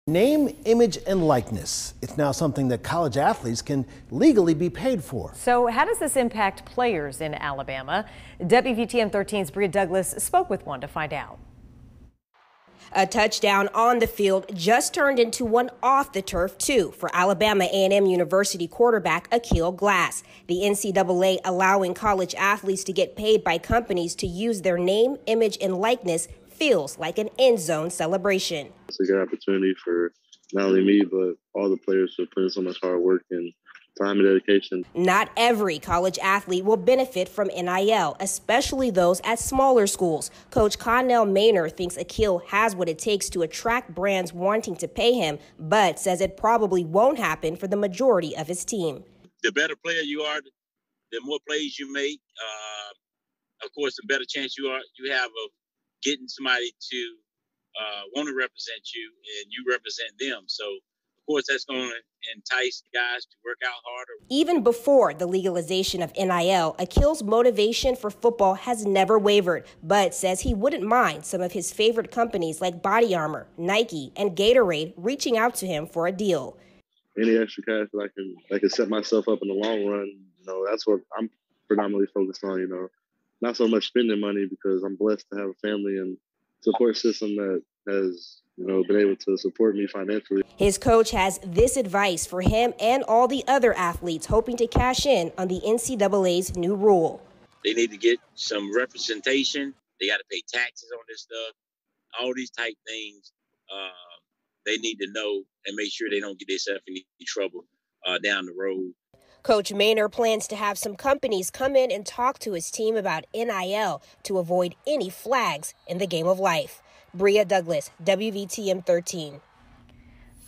Alabama A&M football player calls NIL money long overdue
Name, image, and likeness is now something college athletes can legally be paid for. Alabama A&M University football player, Aqeel Glass calls it a blessing. Watch the video above to learn more.